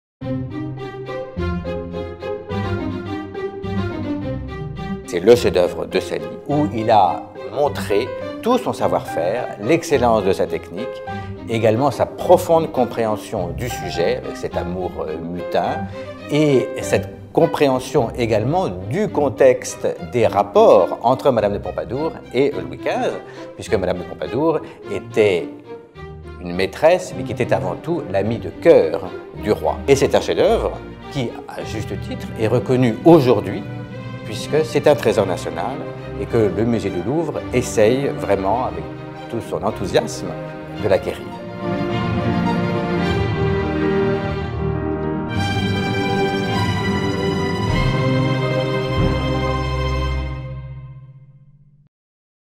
Le chef-d’œuvre de Jacques Saly
Devenez mécène de l'Amour ! Participez à l'acquisition d'un chef-d'œuvre de la collection de la marquise de Pompadour ☛